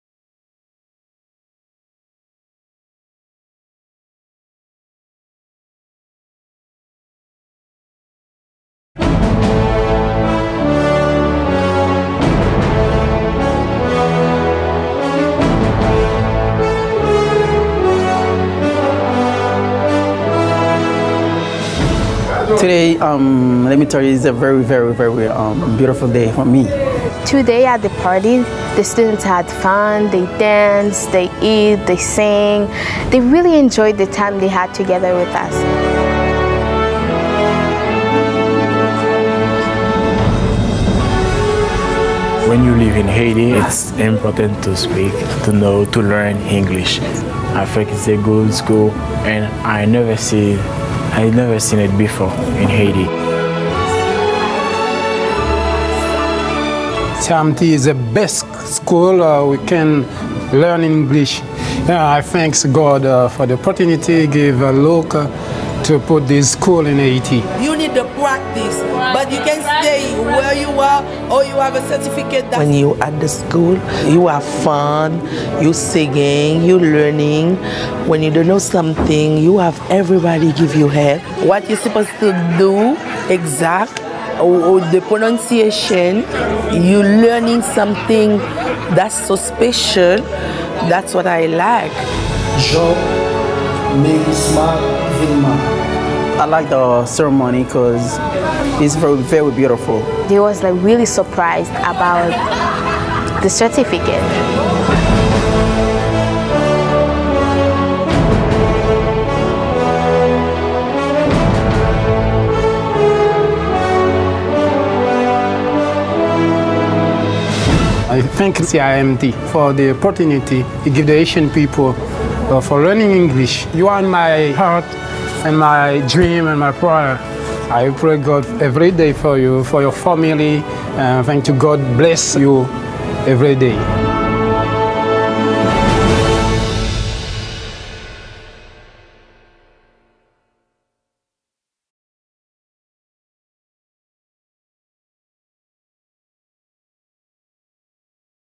Haiti - CIMT Certification Ceremony
Watch and smile as the first wave of students from The Caribbean Institute of Media Technologies recieve their certificates of completion.